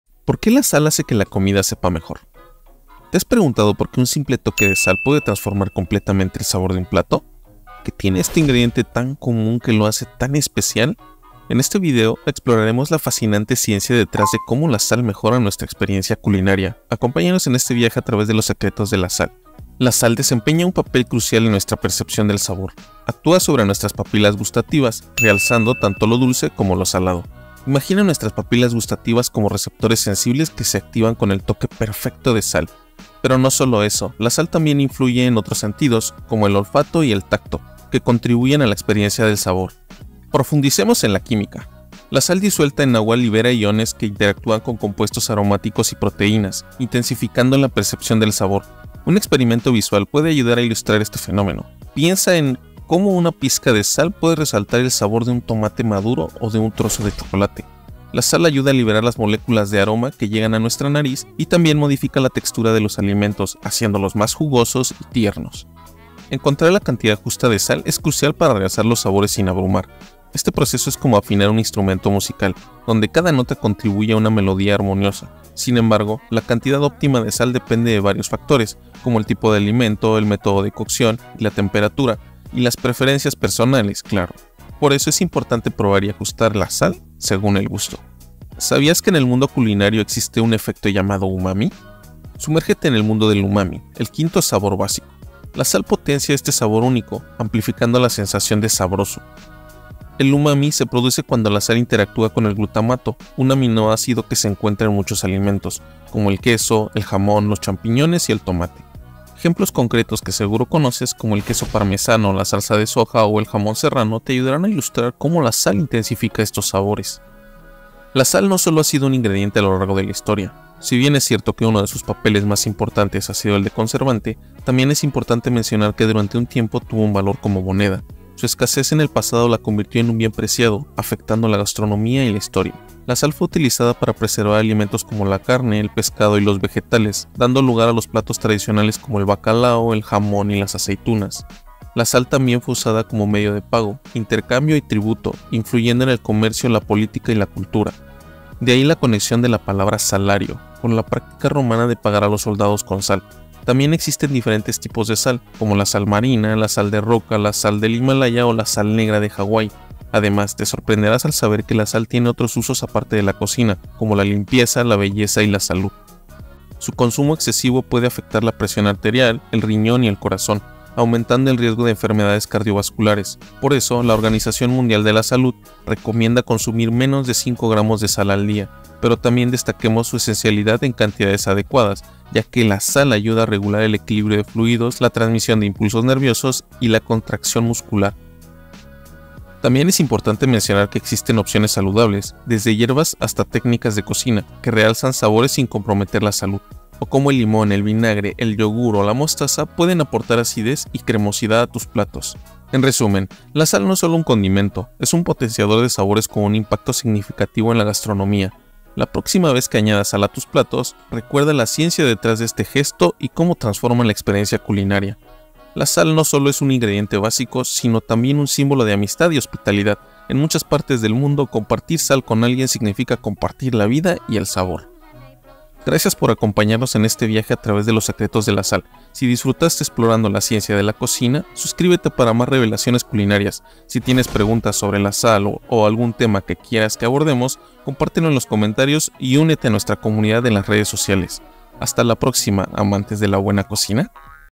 ¿Por qué sabe mejor?
Sumérgete en el mundo de los sabores mientras exploramos el misterio detrás de por qué la sal mejora el gusto de nuestros platillos favoritos. En este episodio, desvelaremos la ciencia detrás de cómo la sal potencia y realza los sabores en la comida. Desde su impacto en las papilas gustativas hasta su papel esencial en la cocina, descubrirás cómo este modesto condimento tiene un impacto sorprendente en nuestra experiencia culinaria. Acompáñanos en un viaje gastronómico que desentraña el encanto de la sal en nuestra cocina diaria.